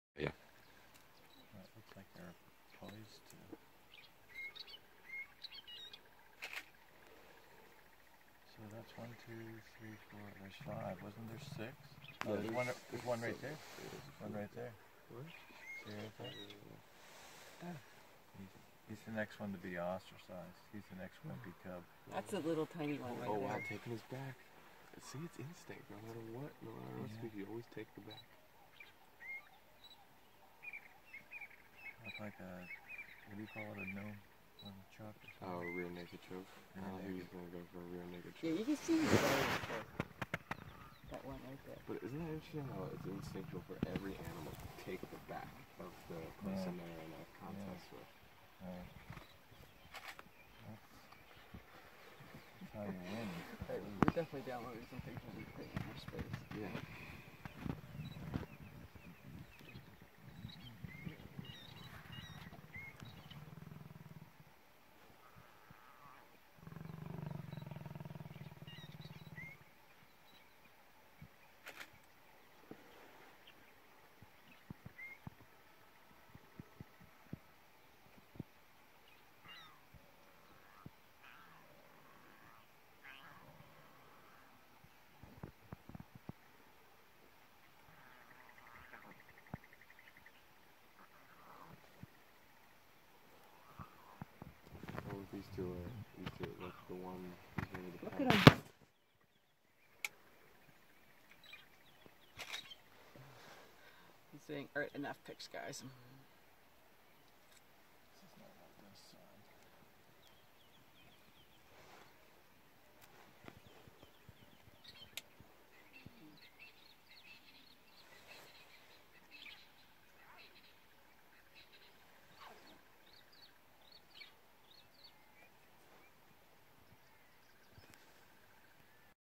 Lion cubs in riverbed
We came around a bend in the riverbed when suddenly we saw this pride of lions playing in the riverbed. Sitting with them for hours just watching them playing and teasing each other.